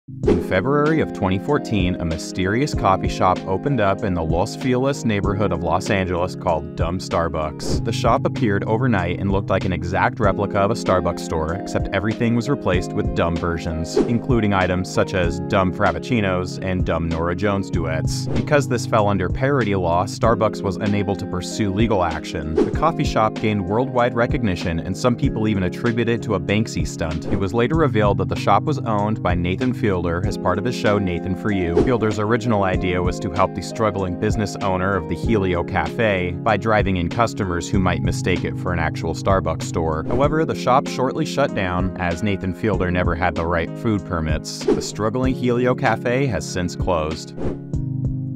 Nathan For Brew: DUMB STARBUCKS Unveiled!🎥☕
Uncover the hilarious mystery of Dumb Starbucks, the coffee shop that appeared overnight in Los Feliz, LA. Explore the parody law loophole, Nathan Fielder's involvement, and the quirky saga that took the coffee world by storm! ☕😂